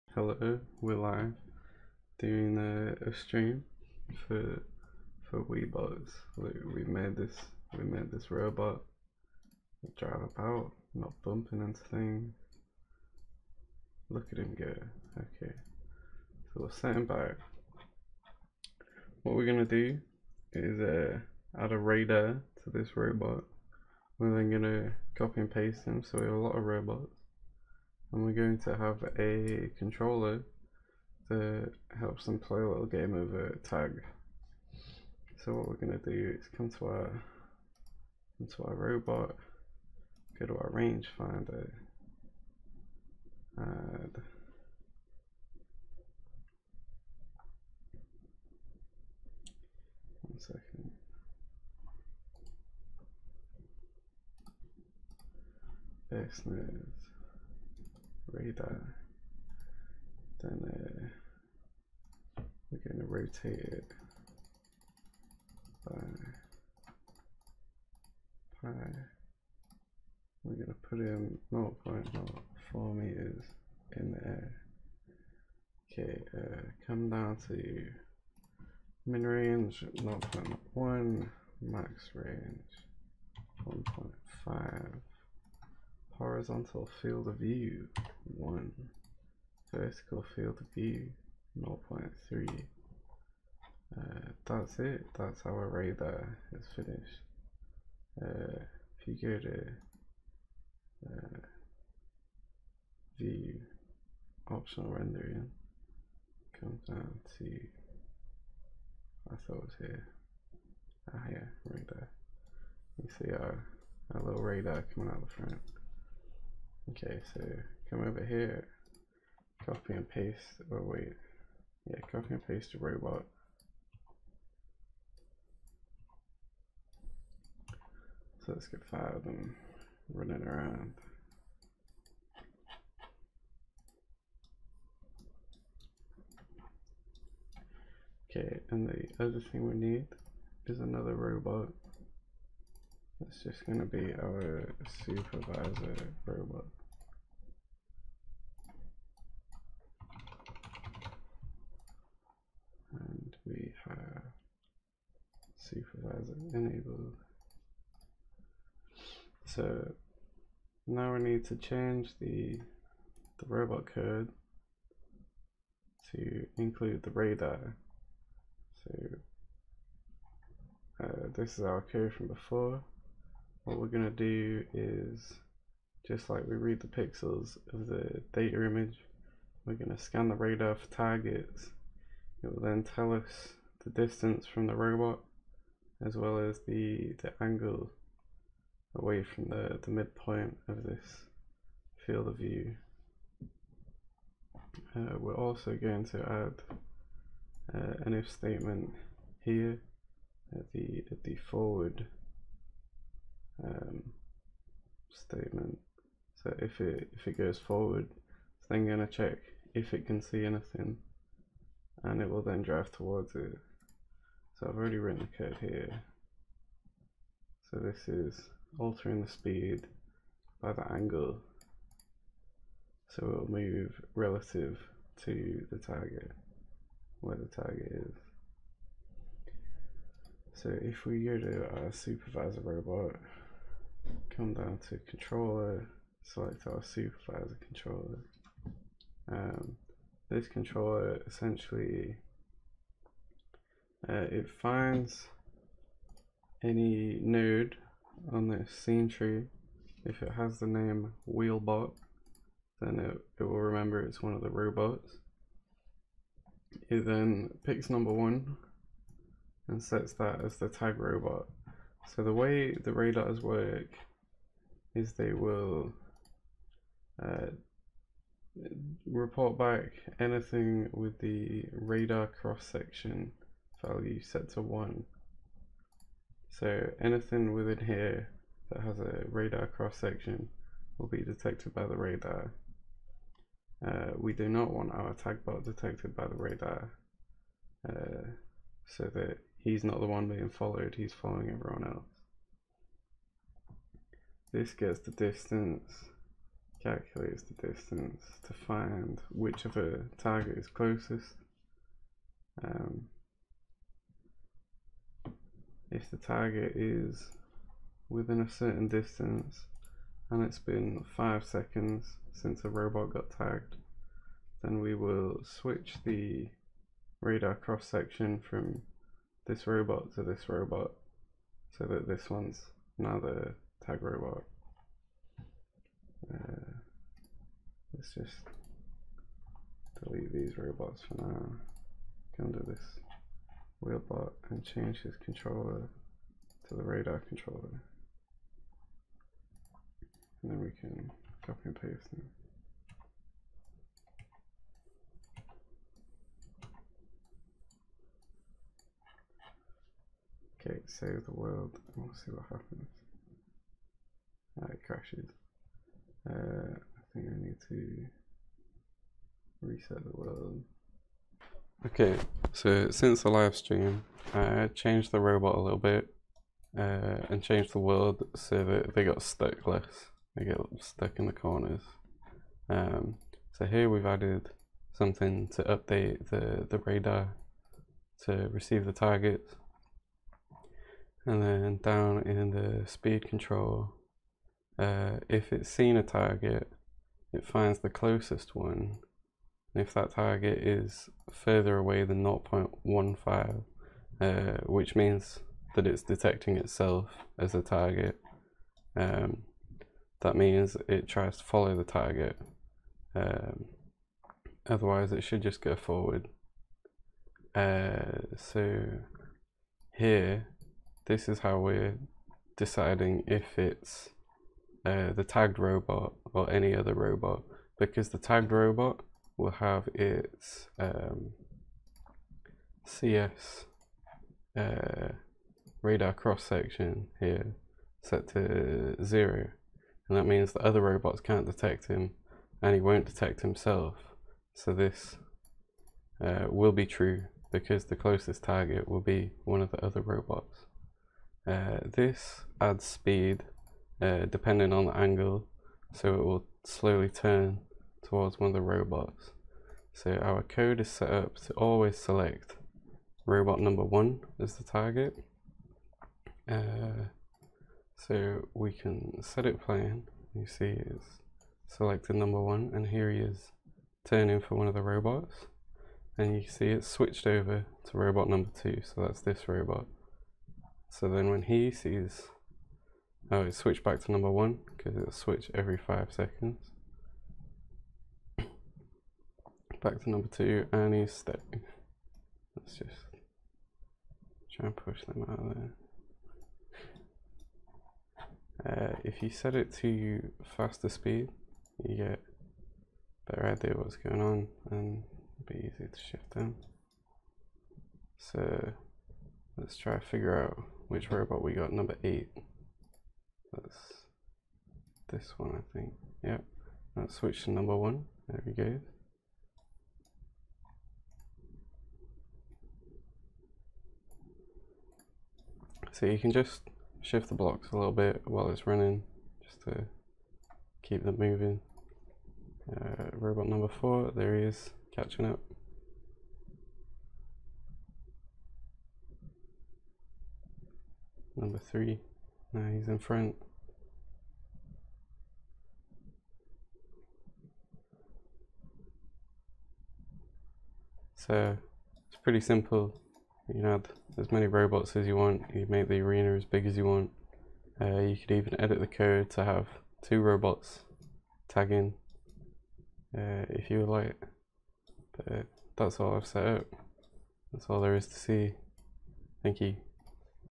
Webots 101: Radar Tag Robots
In thie tutorial we add a radar to our previous robot so that it can find and follow other robots.